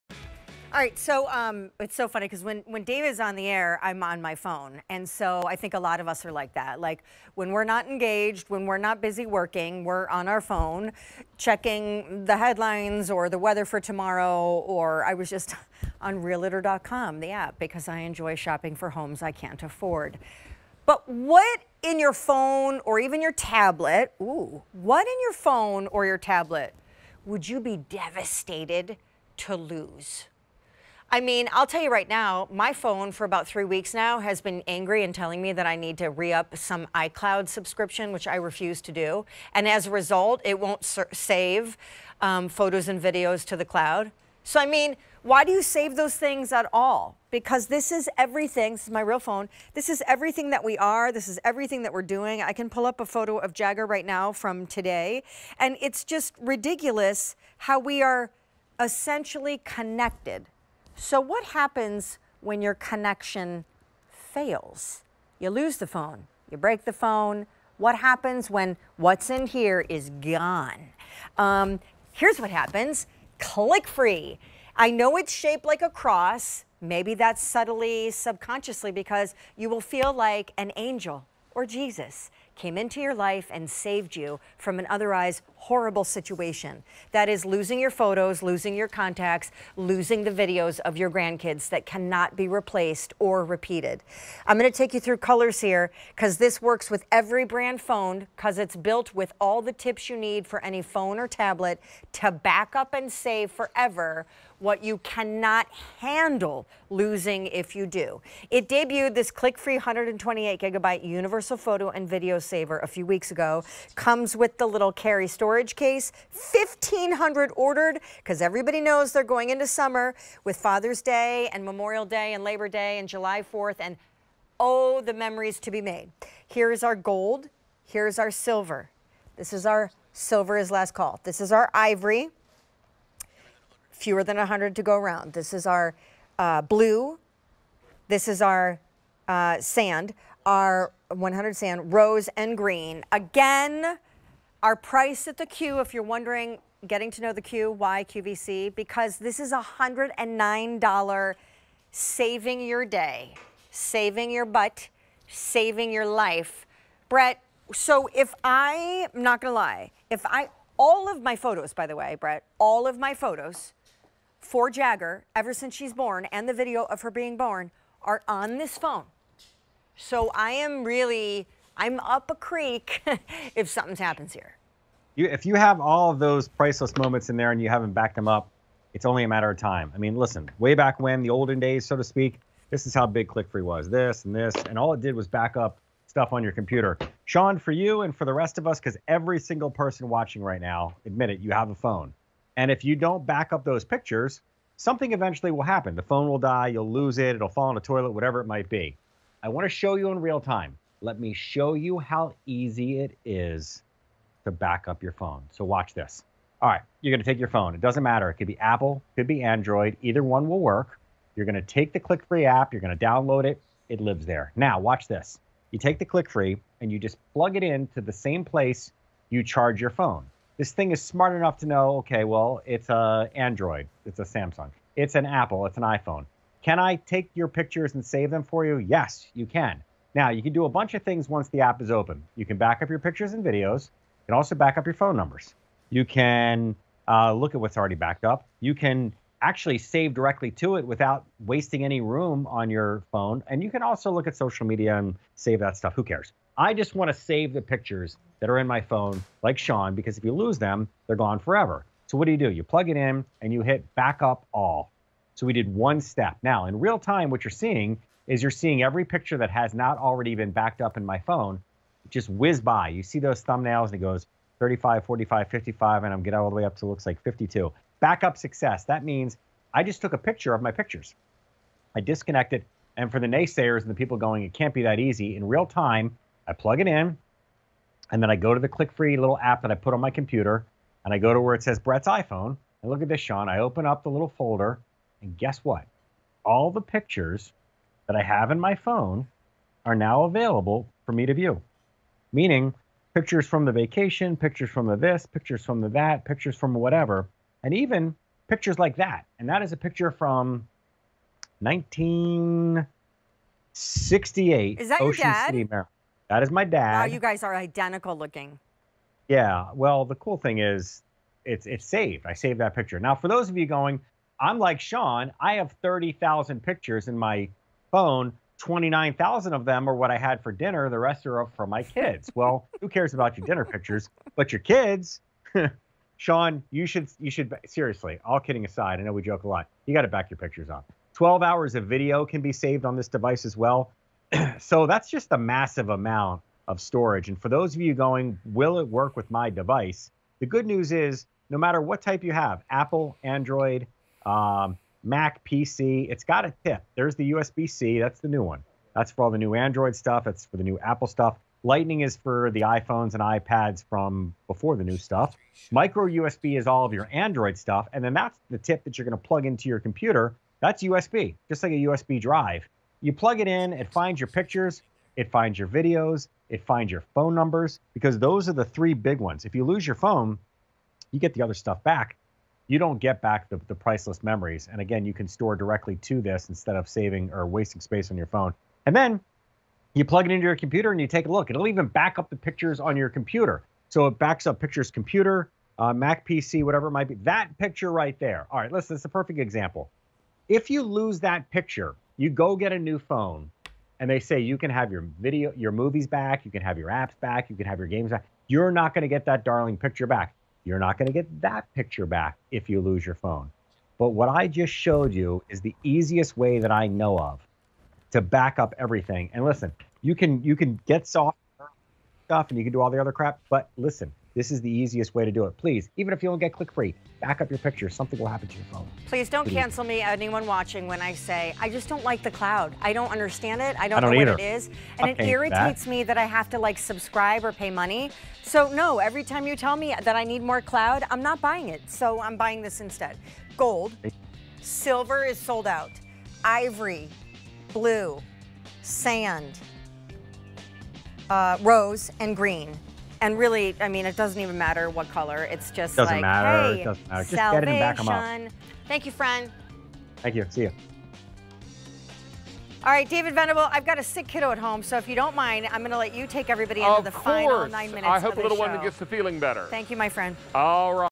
ClickFree 128GB Universal Photo & Video Saver with Travel Carry Case on QVC
ClickFree 128GB Universal Photo & Video Saver with Travel Carry Case

A simple and smart solution for your storage woes, the compact and convenient ClickFree Backup Genius features four connectors so you can easily expand the capacity of any device -- fitting more photos and videos on your phone, tablet, or computer without worrying about running out of space. Plus, you'll also receive a travel carry case.